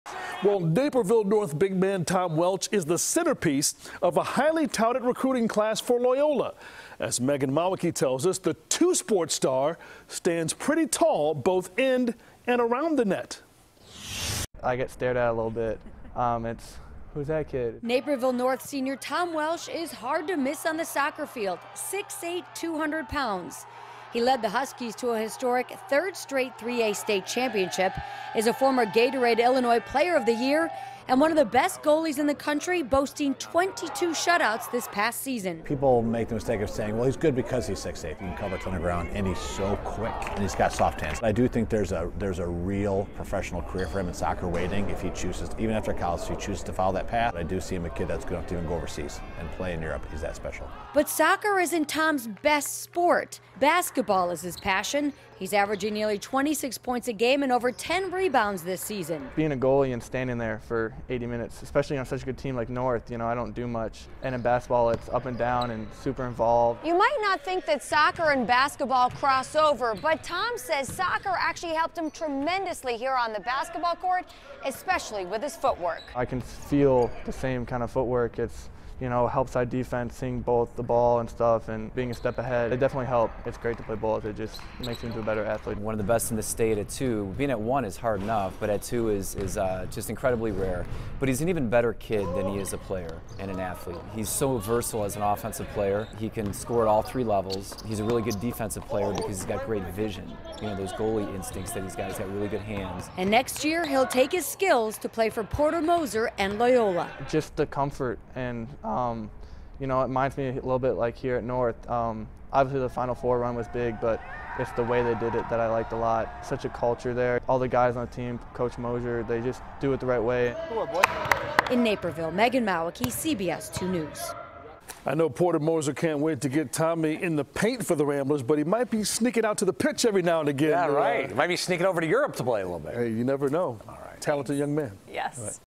Naperville North Sports Star Heading To Loyola
Naperville North big man Tom Welch is the centerpiece of a highly-touted recruiting class for Loyola.